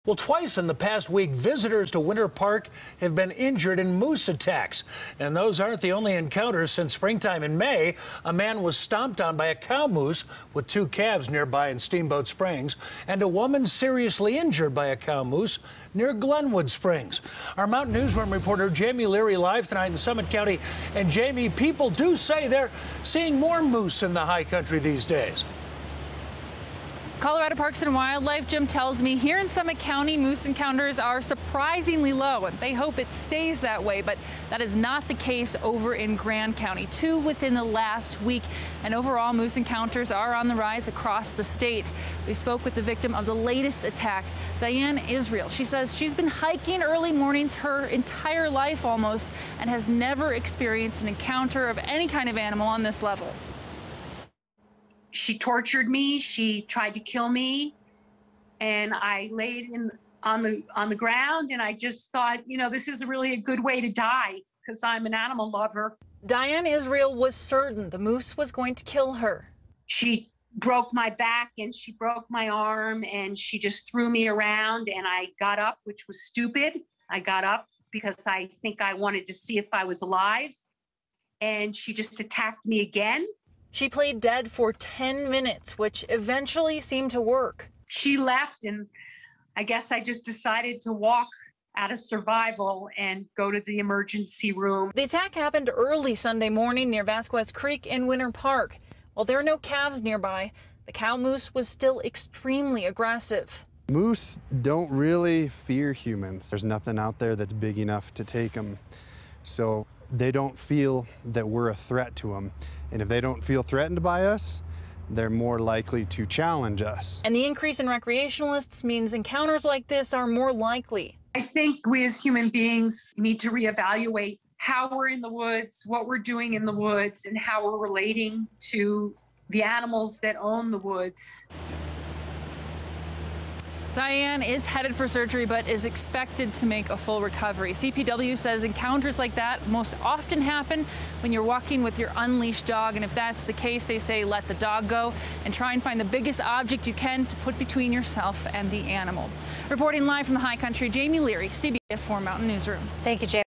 Moose Encounters With Humans On The Rise In Colorado's High County
Twice in the past week visitors in Winter Park have been injured in moose attacks.